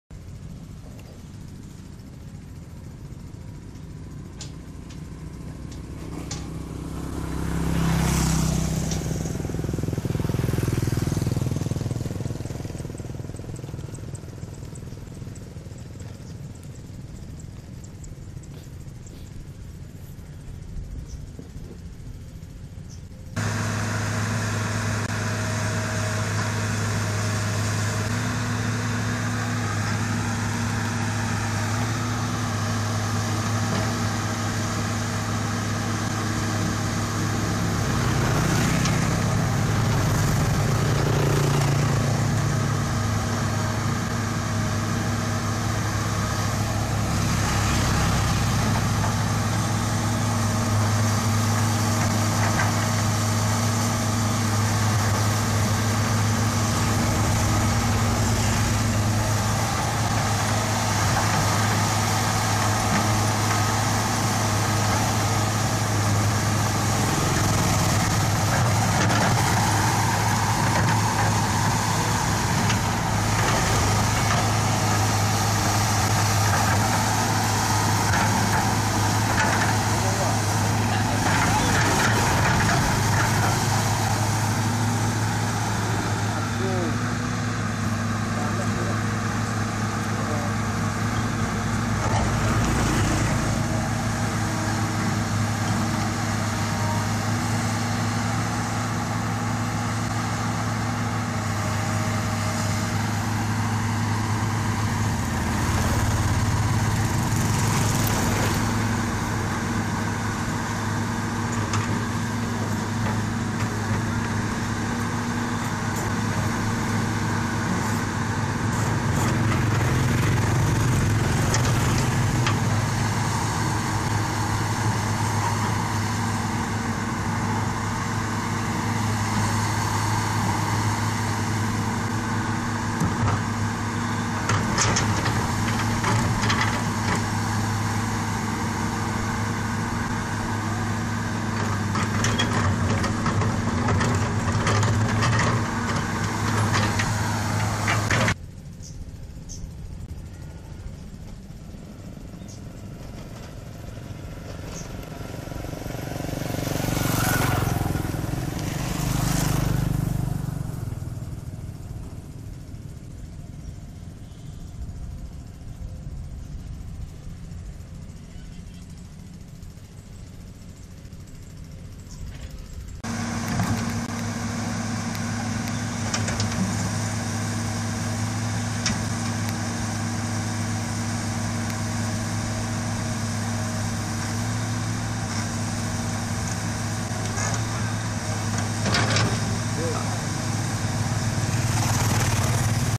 PERBAIKAN KALI ATAU SUNGAI DI DAERAH CIREBON (Desa Kejiwan Kec Susukan)
Perbaikan kali atau sungai supaya aliran sungai tetap bersih dan rapih dan sungai tetap mengalir airnya sehingga para petani tidak kekurangan air.